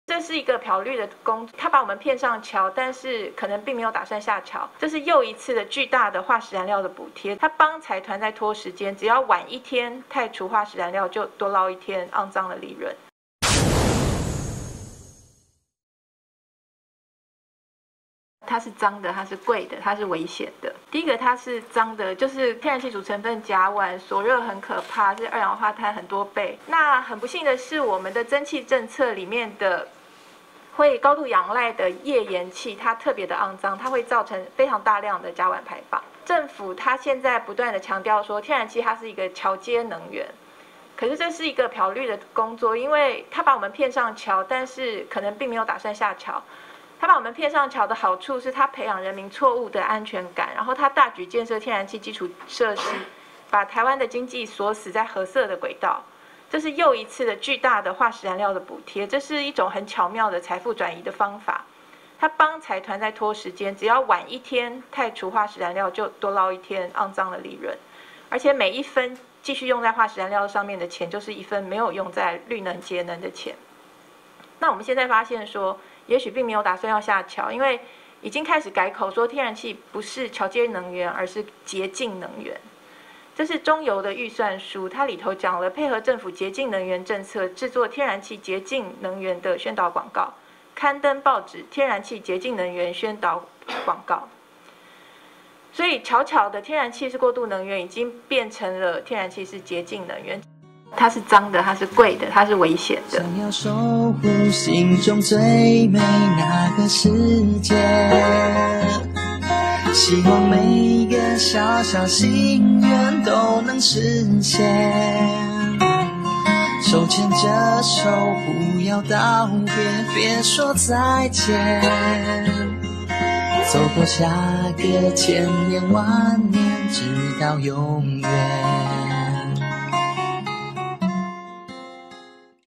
買廣告漂綠？天然氣從橋接能源變潔淨能源？政府在幫財團賺骯髒的利潤？為什麼天然氣是髒的？是貴的？是危險的？你不知道的真相！
買廣告漂綠？天然氣從橋接能源變潔淨能源？
政府在幫財團賺骯髒的利潤？
為什麼天然氣是髒的？是貴的？是危險的？
你不知道的真相！